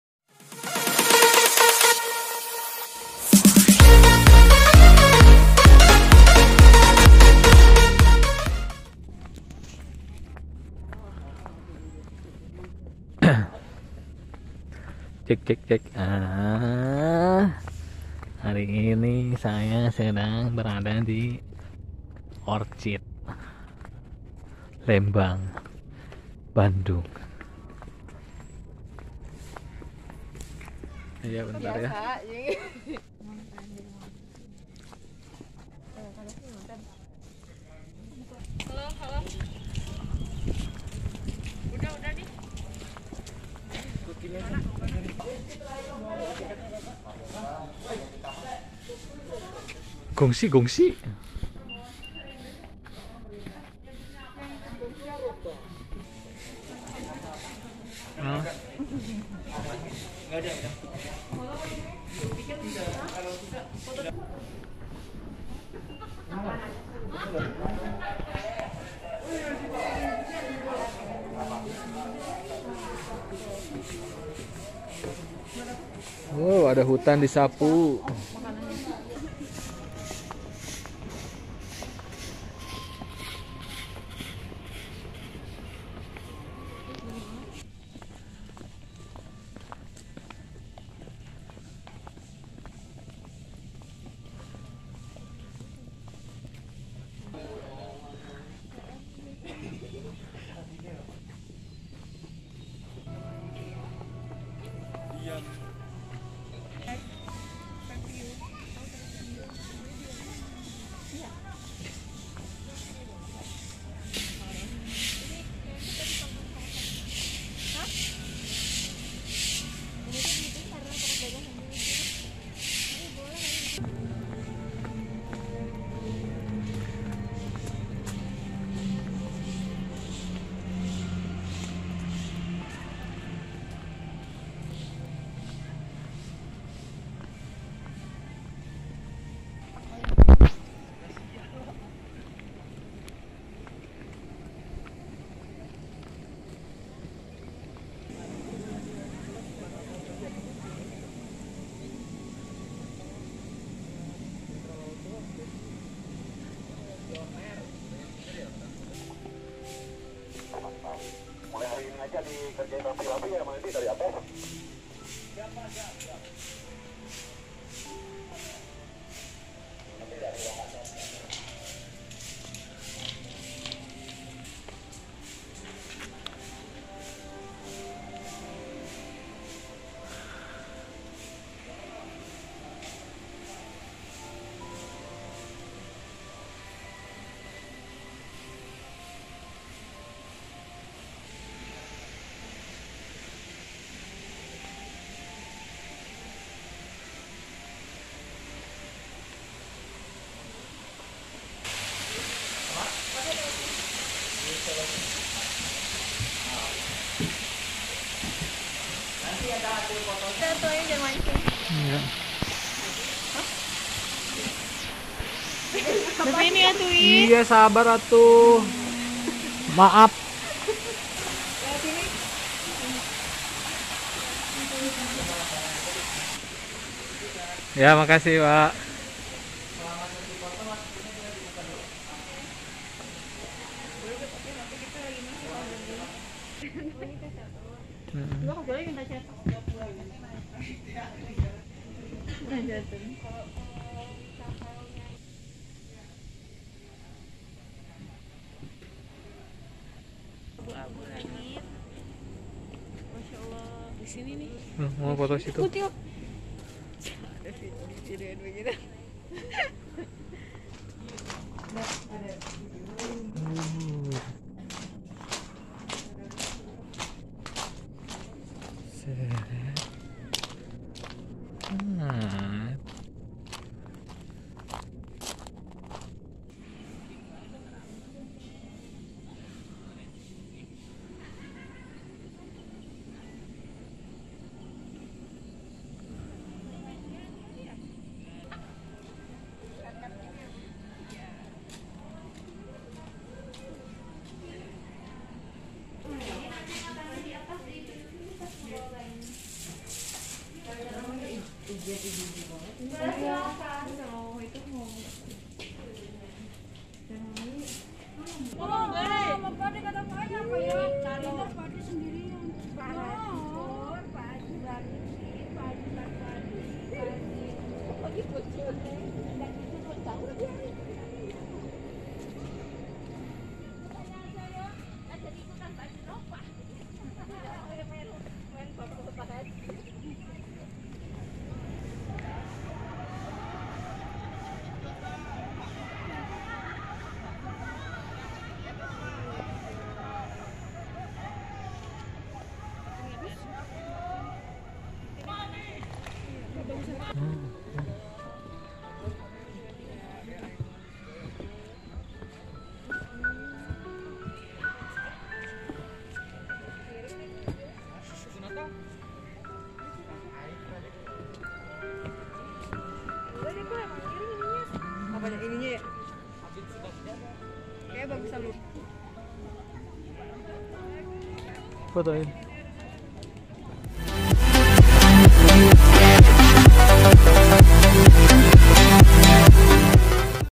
REVIEW HARGA TIKET DAN SUASANA DI ORCHID FOREST LEMBANG BANDUNG
Harga Tiket weekend Rp. 50.000 per orang, weekdays Rp. 40.000 belum termasuk parkir kendaraan.

Parkir mobil Rp. 10.000
Parkir motor Rp. 7500.

Naik Flaying fox sekaligus nyebrang jembatan bayar lagi Rp. 20.000

Naik kuda Rp. 50.000

Ngasih makan keinci bayar lagi Rp. 8000

Selamat berlibur.